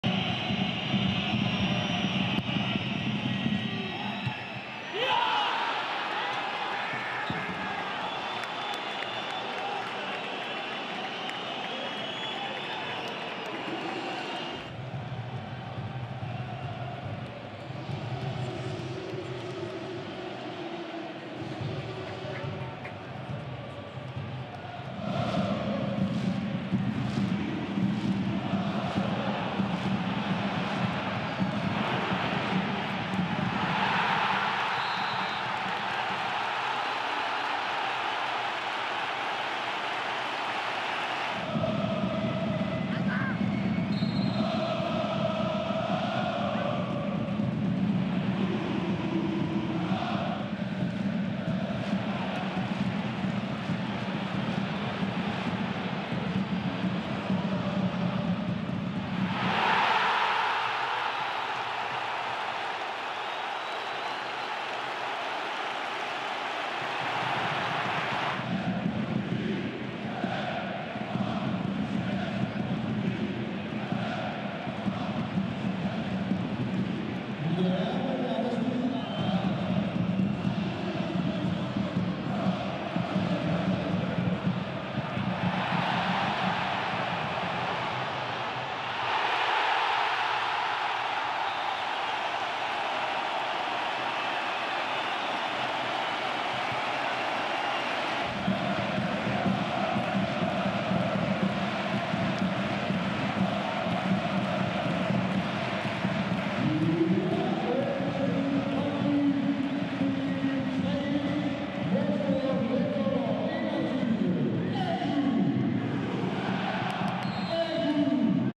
Highlights: Brøndby IF-FC Nordsjælland 3-1
Emiliano Marcondes skaffede FCN en drømmestart, men efterfølgende fejl blev straffet hårdt af et hurtigt omstillingshold som Brøndby.